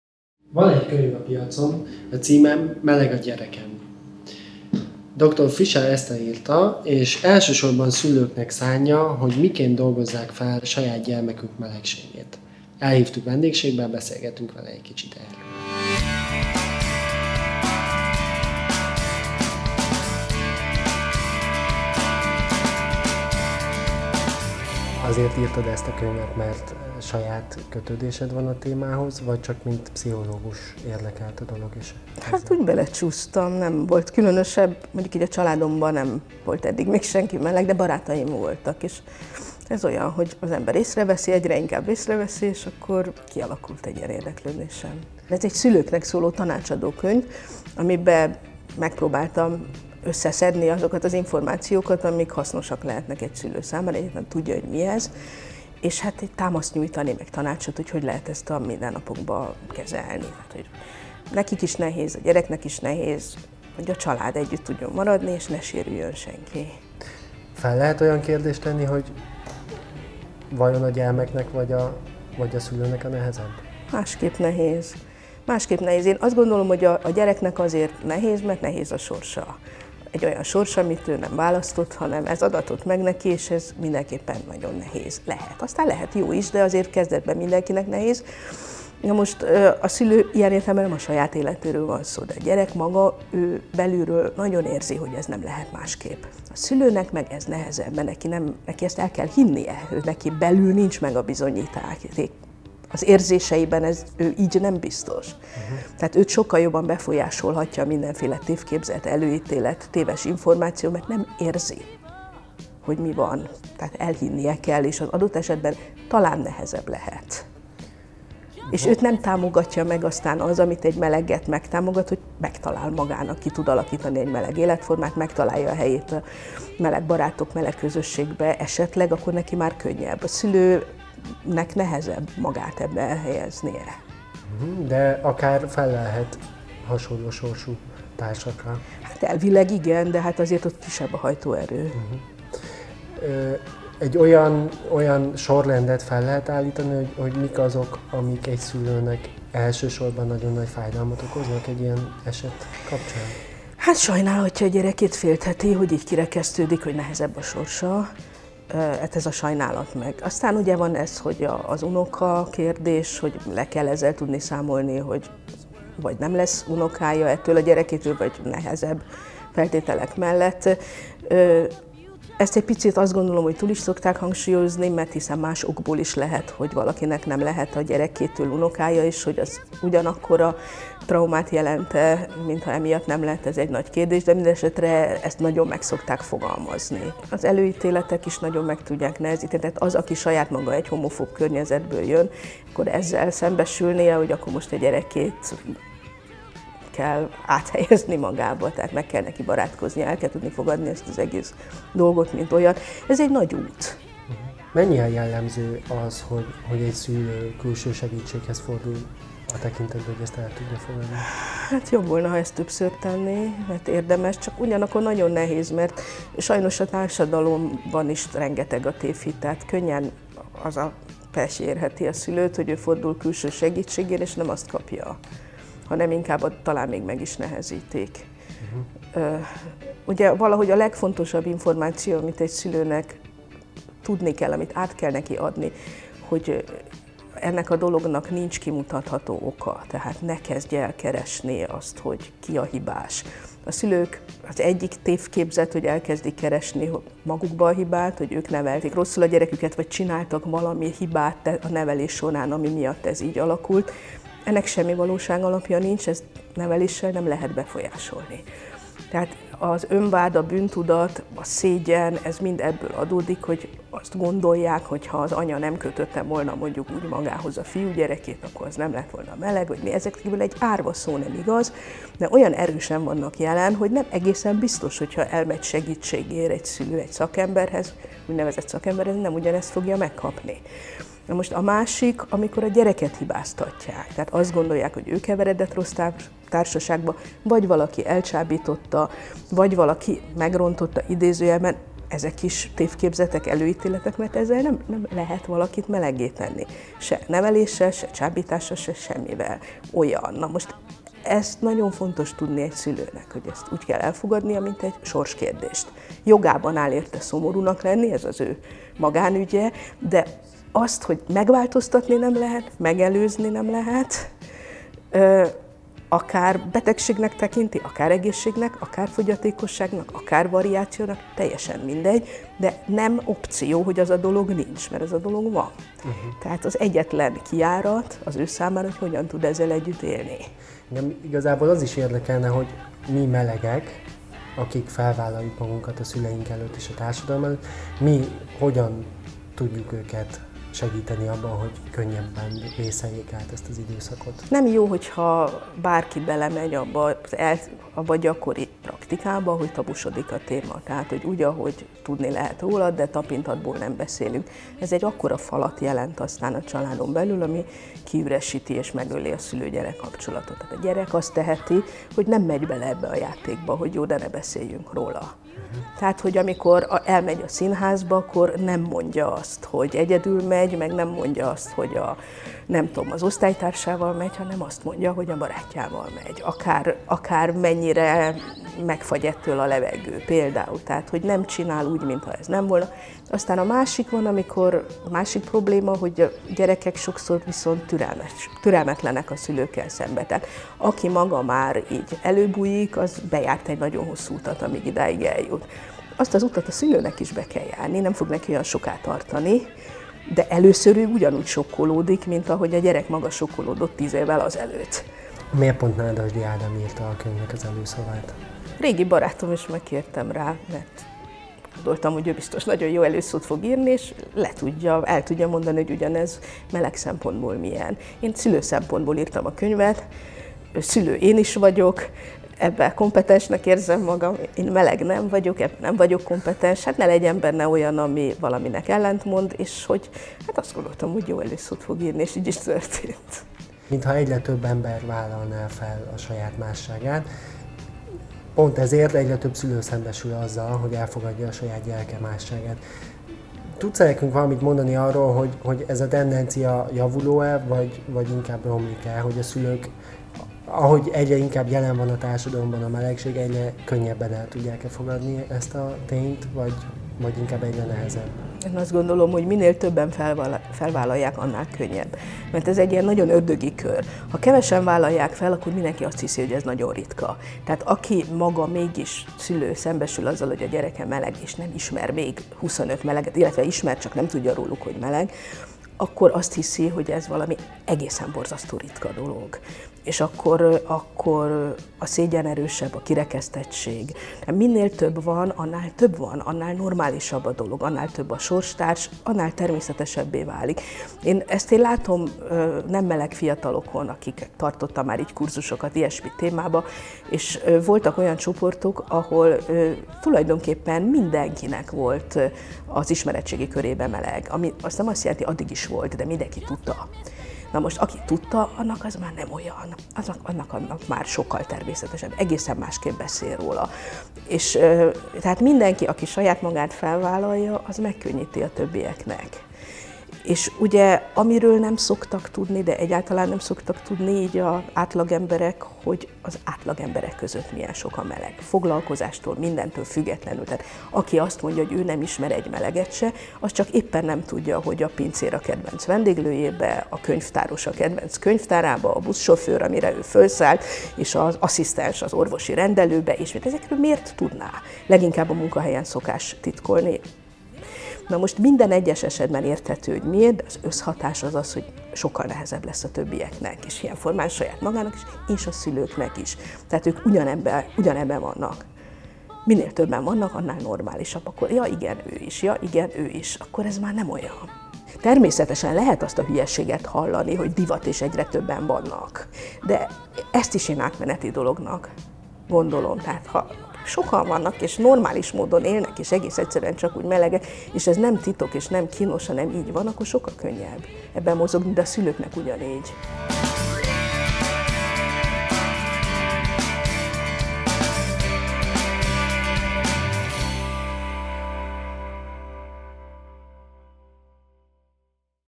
Meleg szemmel 70. - Meleg a gyerekem?!
Dr. Fischer Eszter pszichológus a szülő és meleg gyereke viszonyát elemzi.